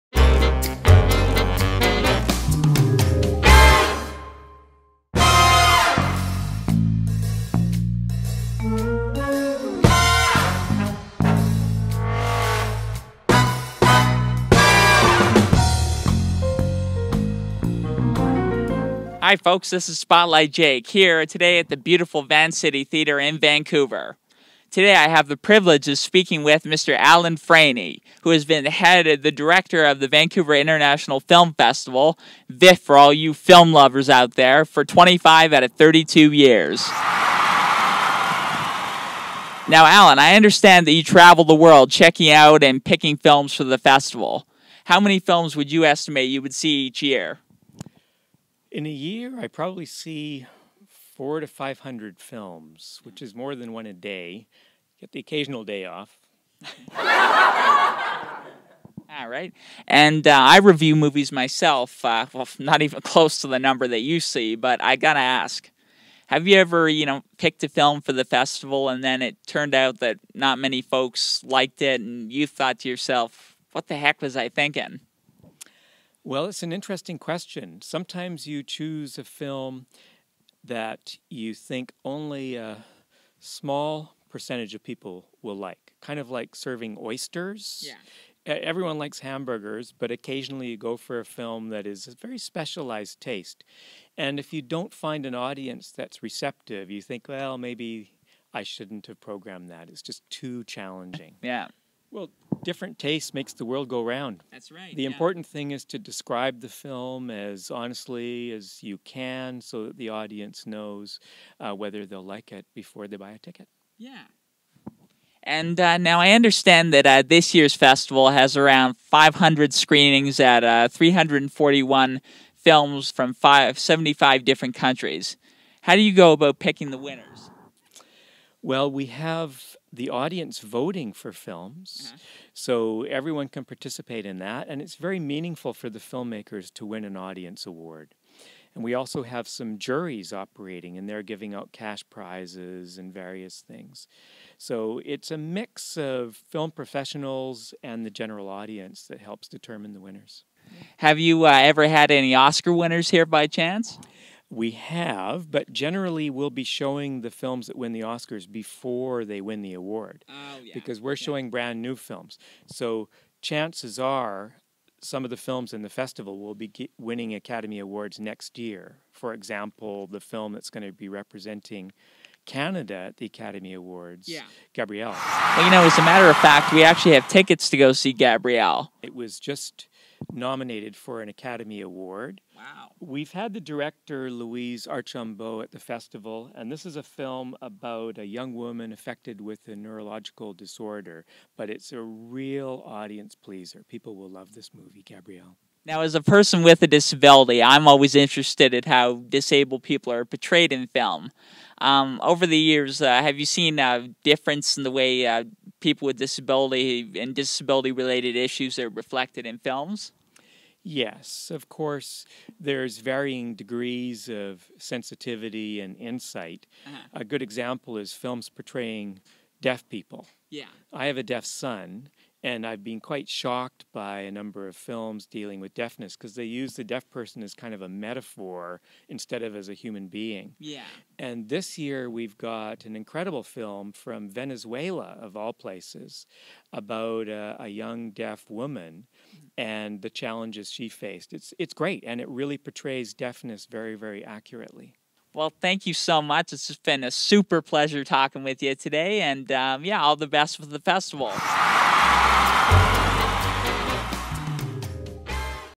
VIFF - Vancouver International Film Festival - a review by spotlightjake
Your review of VIFF includes an interview with festival director Alan Franey,